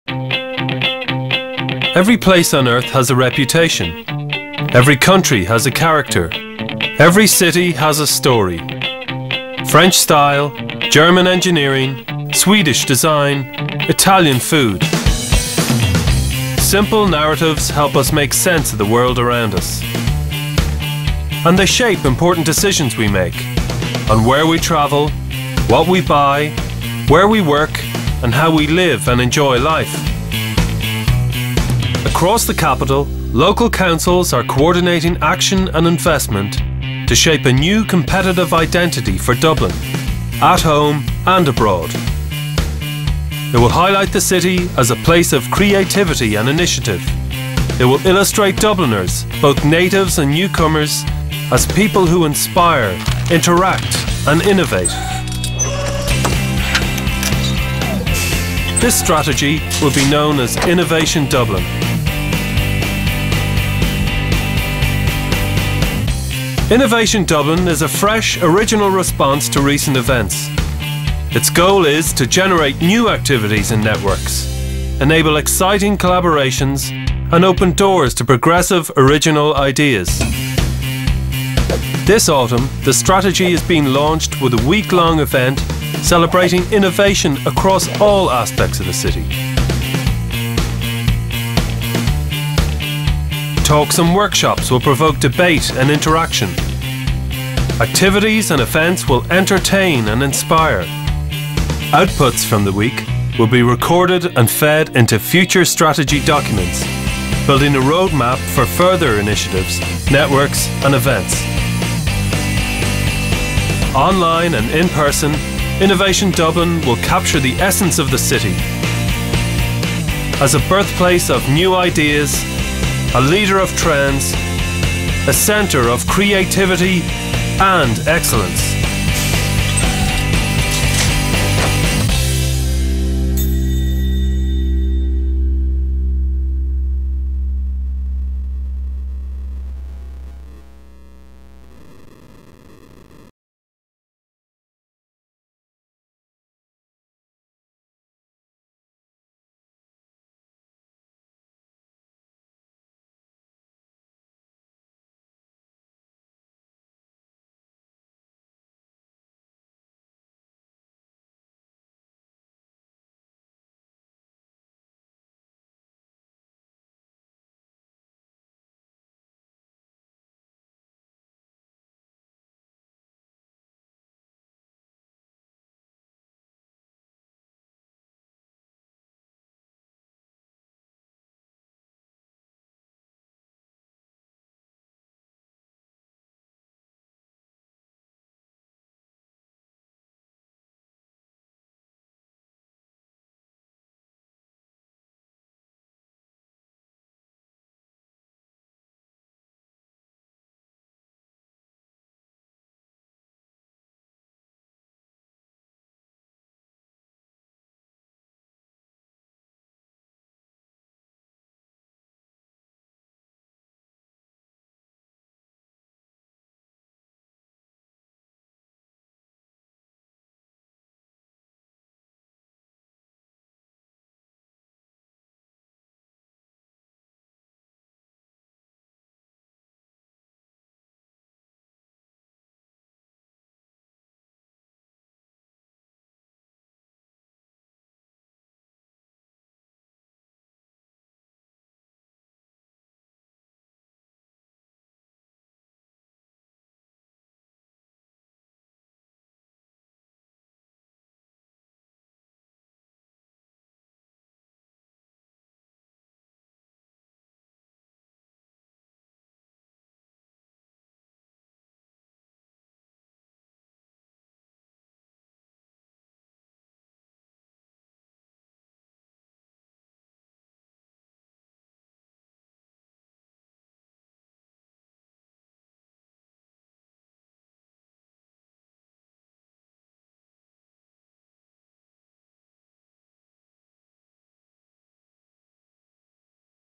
Innovation Dublin Oct 14th - 20th
Innovation Dublin is a week-long festival of workshops, events and showcases highlighting and encouraging innovation in the city.